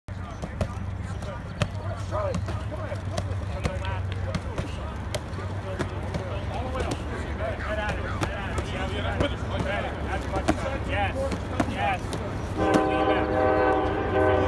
Pitt OL Pass Blocking Drill 4/4/19 | Pitt Spring Practice | PSN
Pitt offensive linemen work with coach Dave Borbely on passing off rushers in a pass blocking drill during spring practice on April 4, 2019.